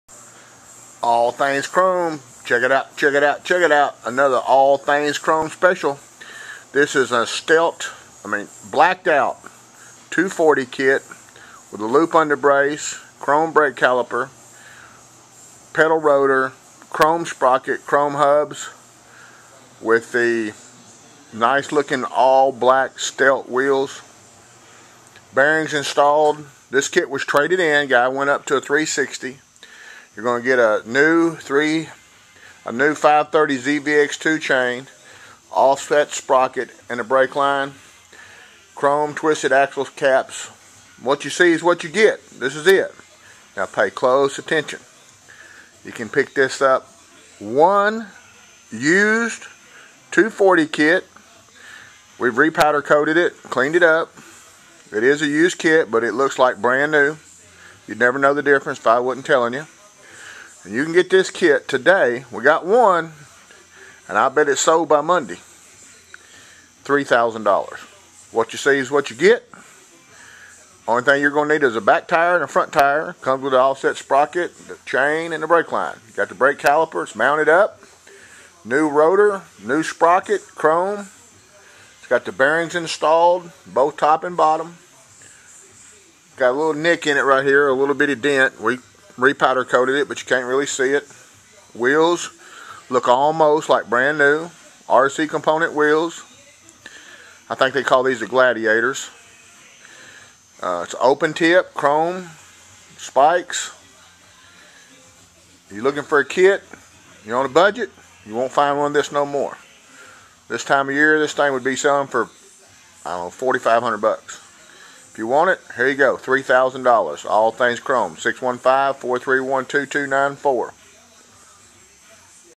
hayabusa 240 sold sold sold super sale price it will not last 3000.00 1 615 431 2294 daniel
super deal on used 240 kit what you see is what you will get
you will need  front and read tire  look at it and listen  what  see is what you will get  put on 3/3 12  7:30 am  thanks           sold sold sold  3/6/12 we can build for you all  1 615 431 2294   it lasted  23 hrs